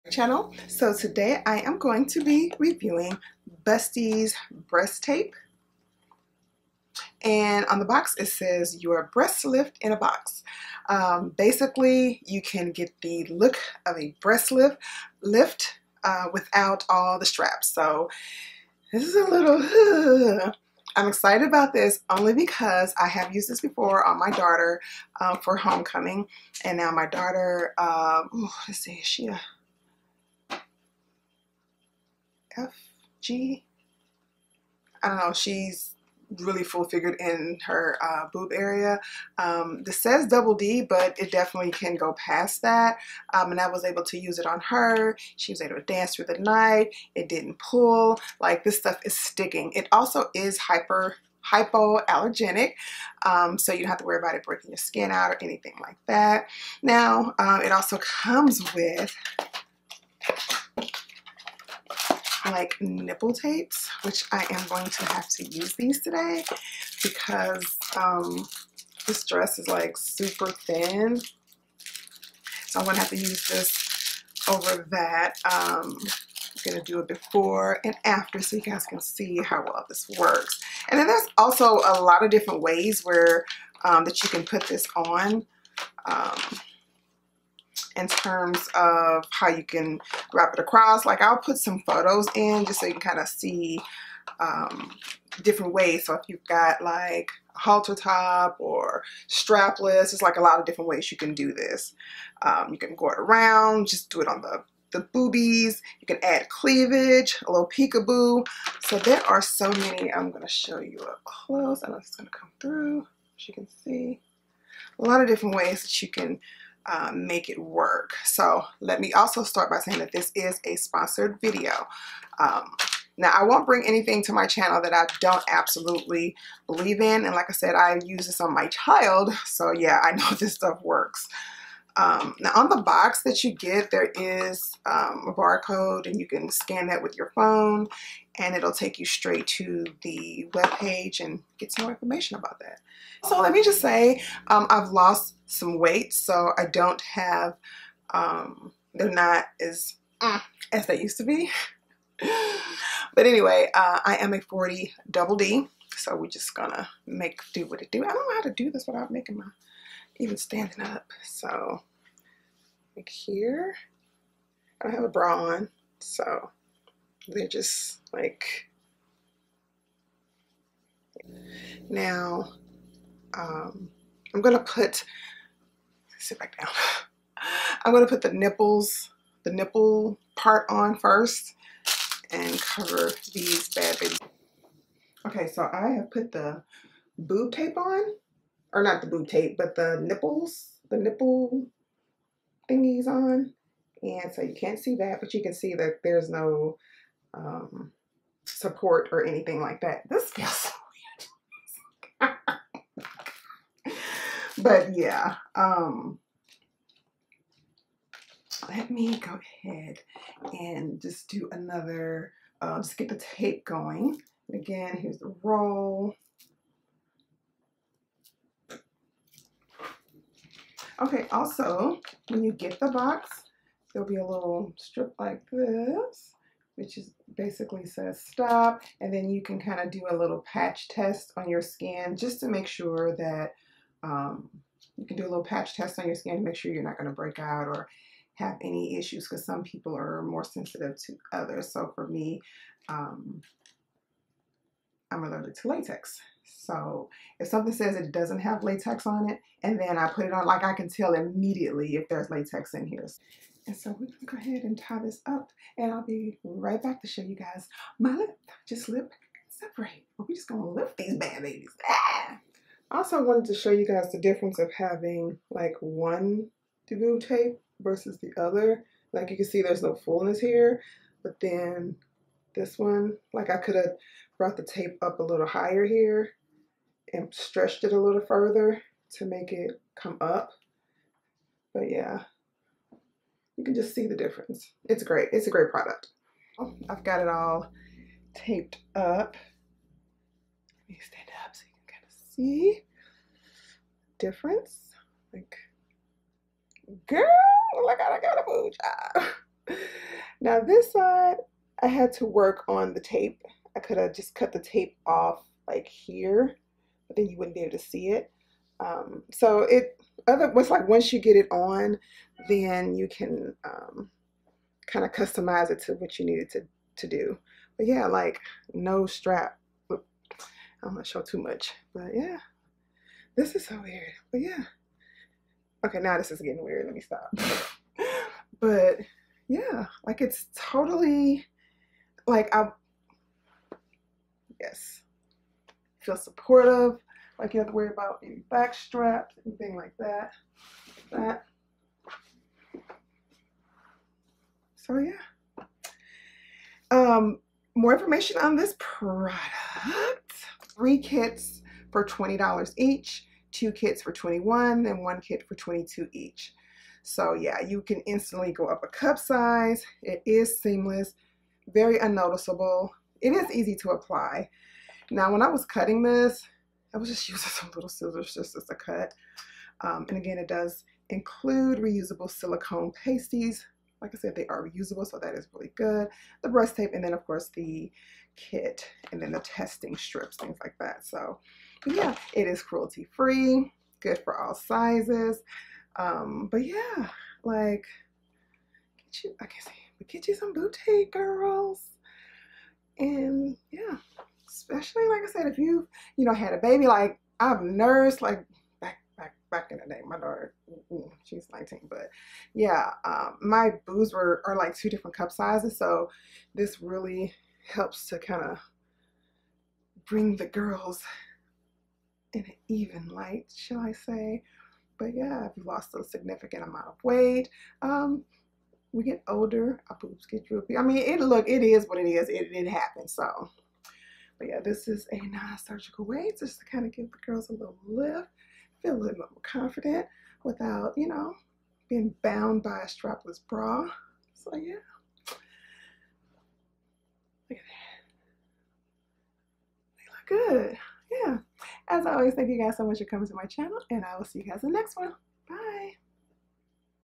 Get Busties breast tape review
This tape is a great addition to add to your wardrobe especially in the summer months or even when wearing an evening gown. Check it out!